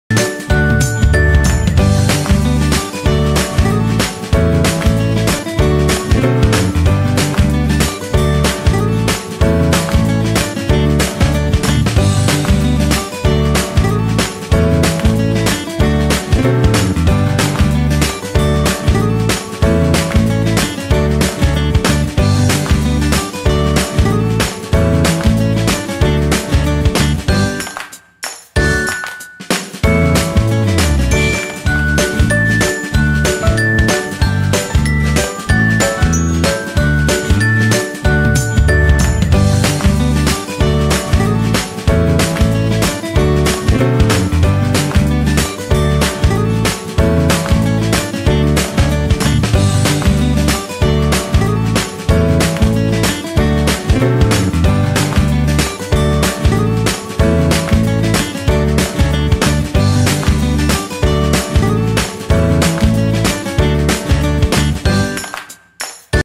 Receita de Pão de Batata Rústico com VONO® Batata com Carne
Assista ao vídeo e confira a receita completa do delicioso Pão de Batata Rústico feito com VONO® Batata com Carne